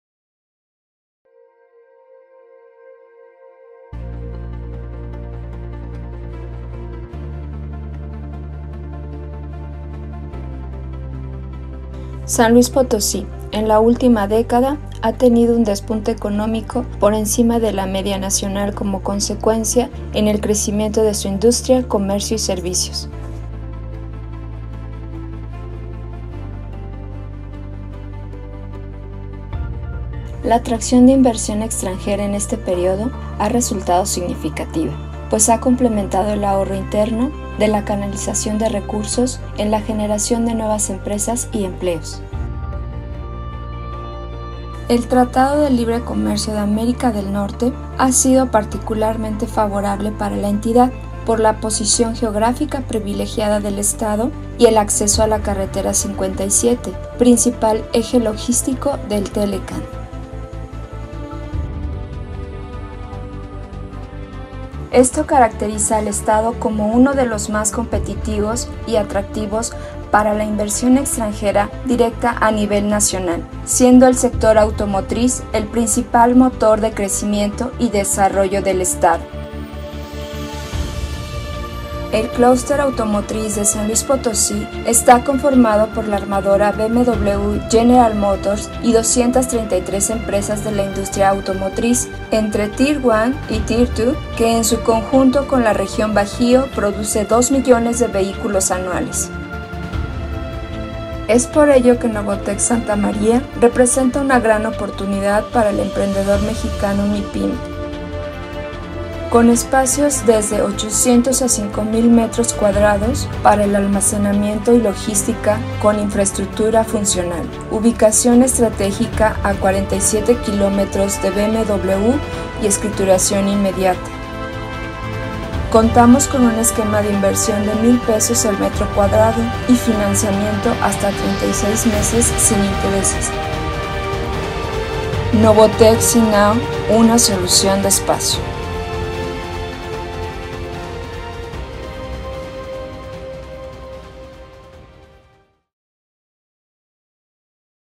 NOVOTECH SANTA MARÍA
TERRENOS INDUSTRIALES

NOVOTECH SI NOW Santa María es un Clúster Industrial con una ubicación diseñada para el Sector Logístico y de Almacenamiento, a media hora de BMW Planta San Luis Potosí y a 50 km de otros parques logísticos. Este proyecto busca integrar al sector comercial, industrial y logístico en una sola identidad, es por eso que en este parque se contará con hotel, comercios varios, áreas recreativas, salas de capacitación, salas de conferencia, gasolinera, entre otros.

Tel Querétaro: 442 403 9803
Tel San Luis Potosí: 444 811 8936